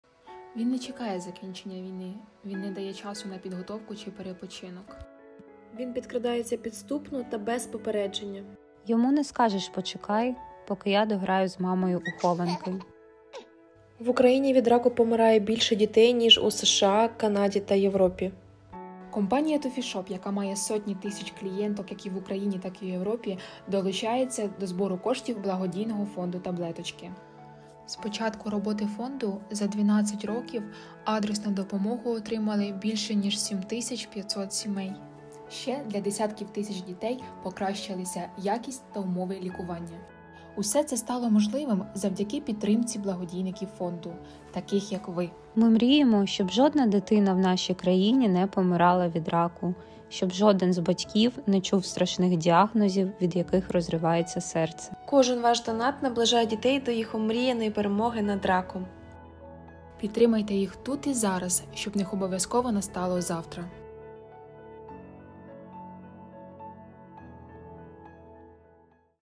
TUFISHOP ДЛЯ МАЛЕНЬКИХ ЧЕМПІОНІВ НАД РАКОМ!
Рак не чекає закінчення війни, не дає часу на підготовку та перепочинок. Він підкрадається підступно та без попередження, йому не скажеш «почекай» поки я дограю з мамою в хованки;
Ми мріємо, щоб жодна дитина в нашій країні не помирала від раку, щоб жоден з батьків не чув страшних діагнозів від яких розривається серце.

Компанія TUFISHOP, яка має сотні тисяч чуйних клієнток в Україні та Європі, оголошує збір на допомогу українським дітям, що борються з раком - підопічним фонду "Таблеточки".

На фото – Дмитрик із Житомирської області, минулого року у його животі виросла велика пухлина, яка охопила важливі органи і судини. Складні операції, хімієтерапія, 28 сеансів променевої терапії, майже місяць у реанімації – лише частина випробувань.
Боротьба Дмитрика триває, адже пухлину поки не вдалося повністю видалити через її складність. Попереду – майже рік підтримувальної хімієтерапії і сподівання на нову операцію. Та поки родина Дмитра радіє перемозі на цьому етапі і поверненню додому. З вашою допомогою такі діти повертаються до звичайного життя.

Підтримаймо комфортною сумою таких діток як Дмитрик, тут і зараз, щоб в них обов’язково настало «завтра».